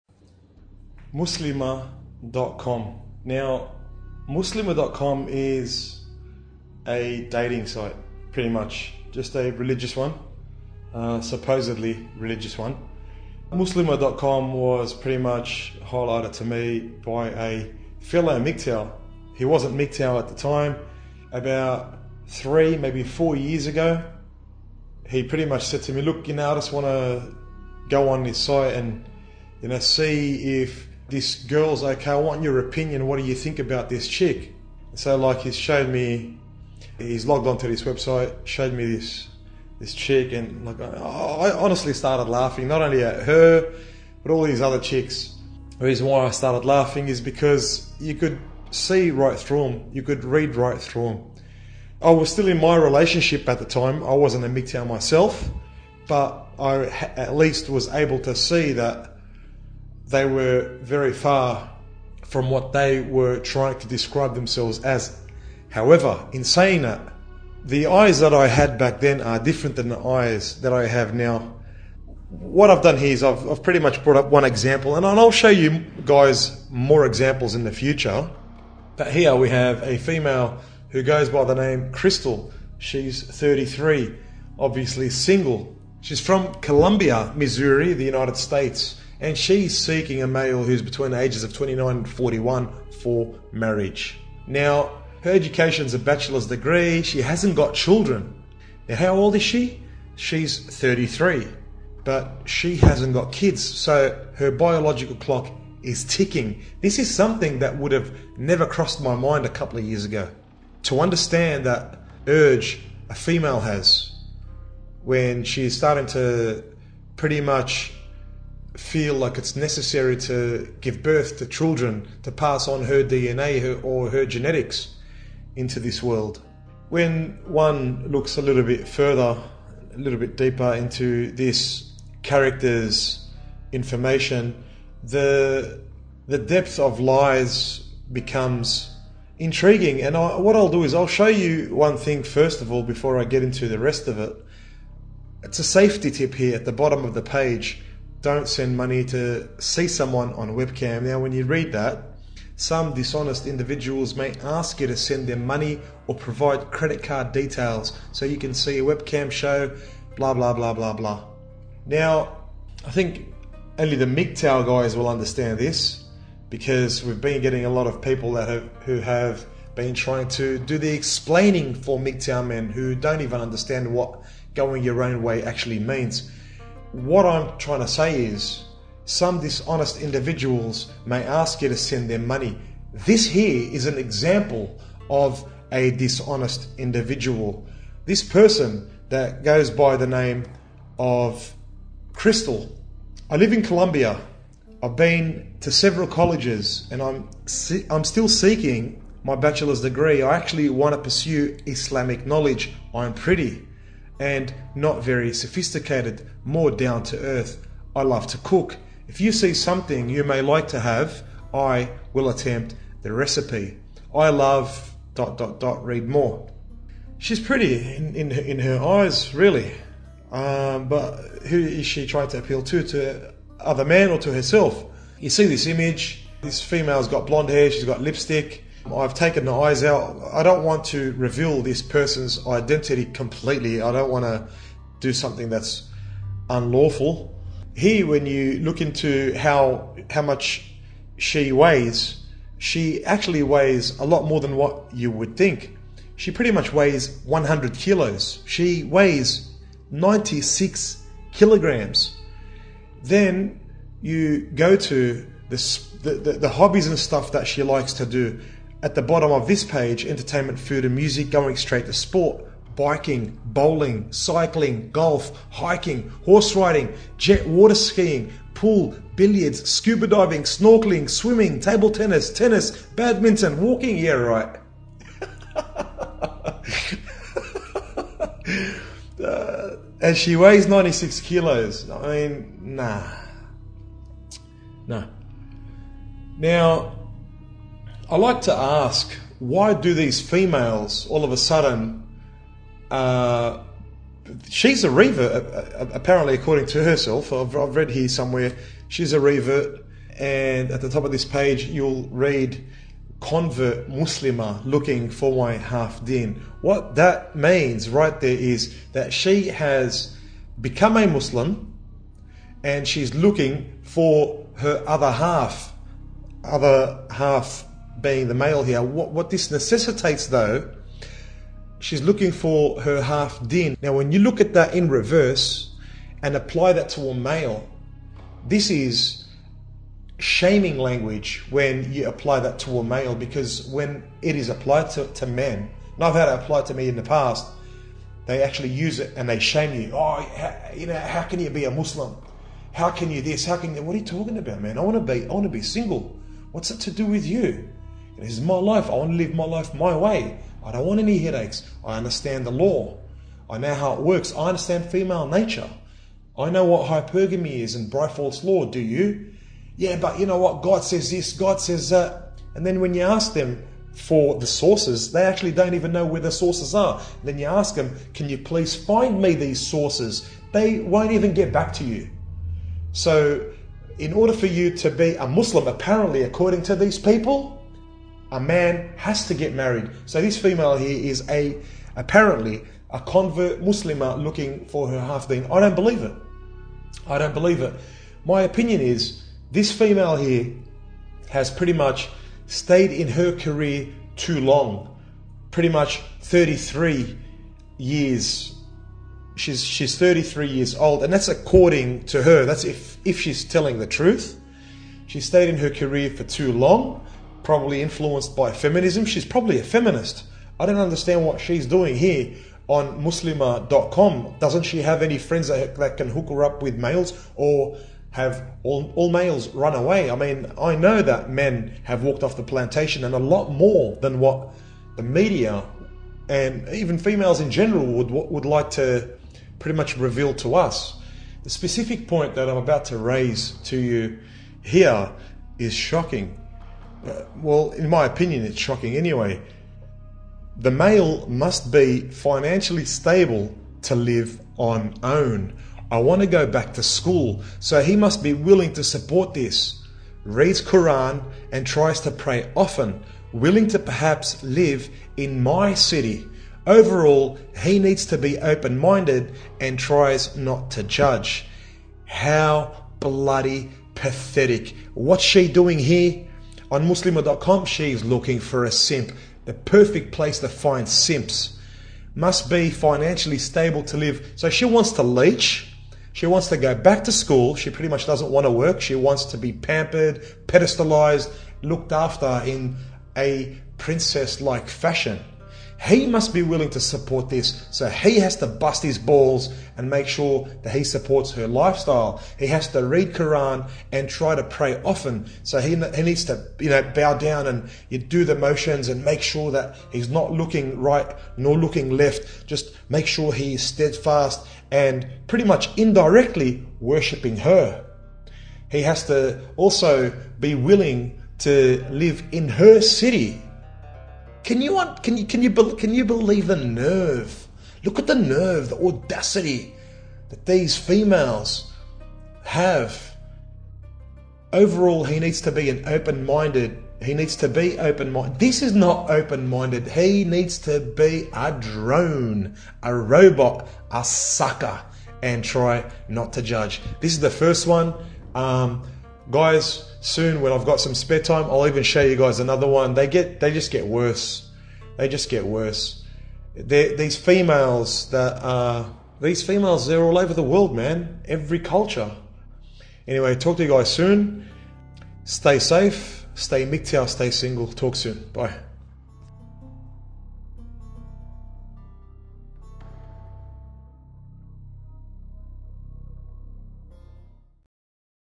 MGTOW - Muslima Dot Simp
Who are these Females trying to kid? Unfortunately there are many Men who will fall for the deceptions of many females. Companies like muslima.com make a profit by allowing Females to lure in Males via lying, manipulation and deception.